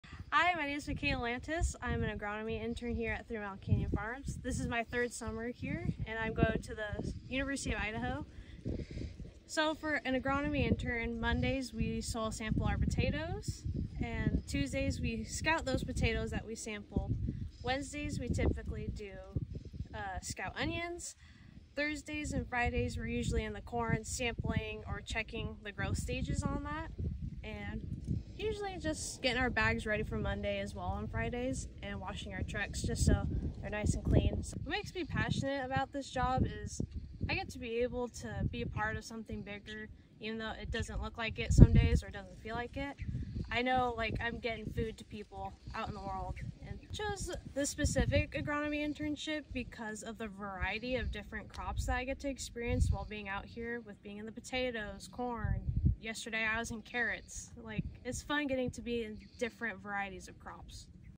Being an Intern at Threemile Canyon Farms: Meet Makaila
Curious what it’s like to be an agronomy intern at Threemile? Meet Makaila, a third-year intern, as she takes us behind the scenes of her week in the fields, working with a variety of crops. For nearly 20 years, the Threemile intern program has been giving college students interested in agriculture hands-on experience.